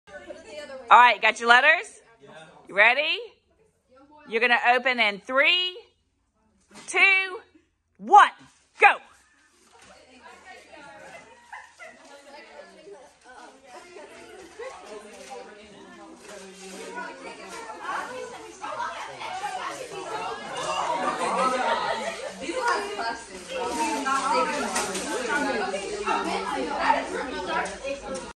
LPS Letter Opening Party!
Watch and listen as students in Mrs. Gerzema's class open letters from their pen pals!